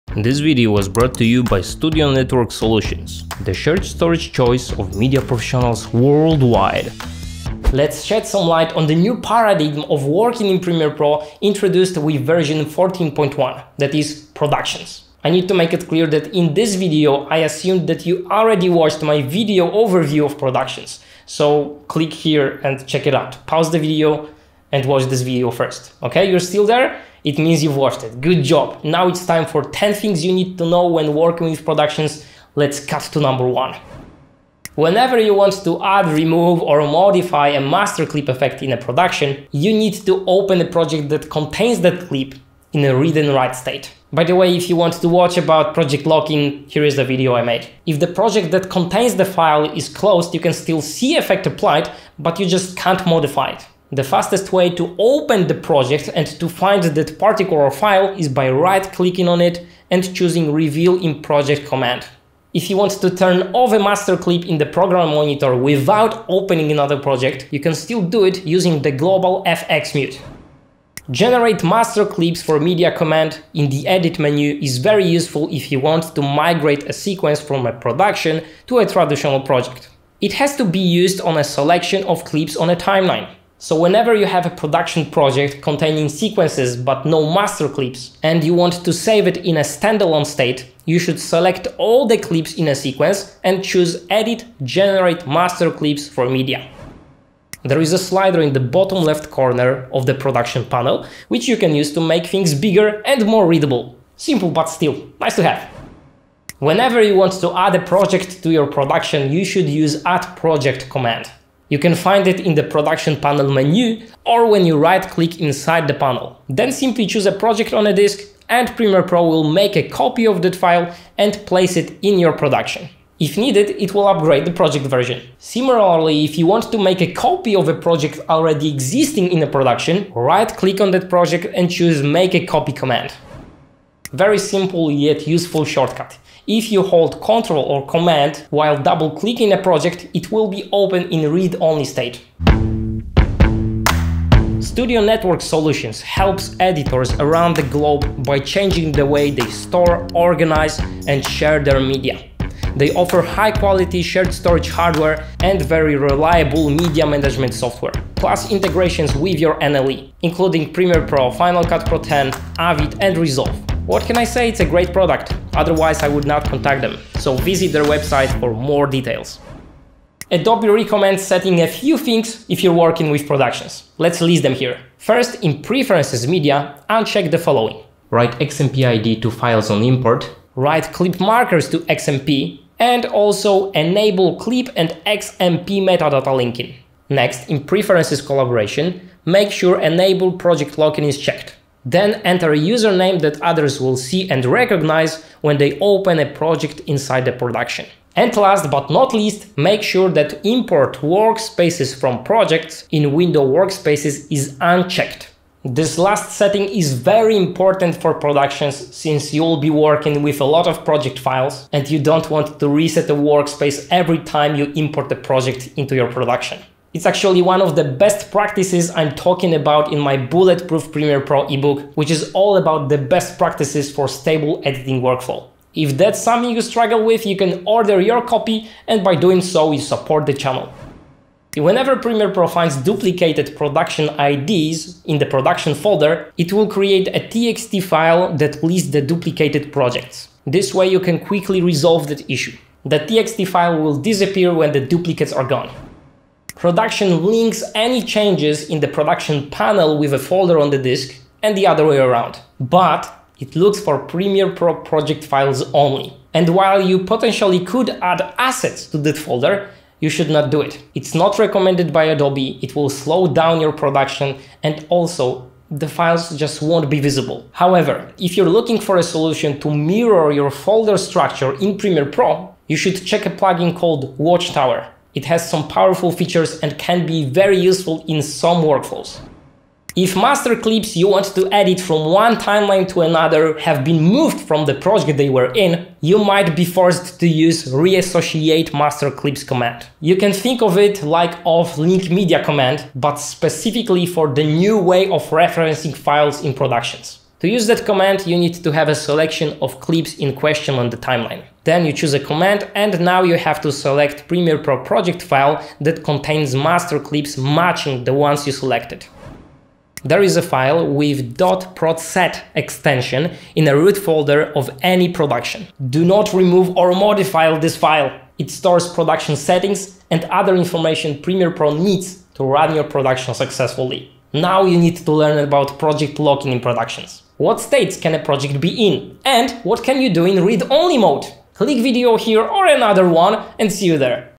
10 TIPS for Using Productions in Premiere Pro
What should you know about when working with Productions Workflow? How to add a project to an existing Production? How to generate master clips for a specific project? How to manage duplicated project IDs and how to open a project in the 'Read Only' state?

▶ Read more about this release of Premiere Pro (work in progress)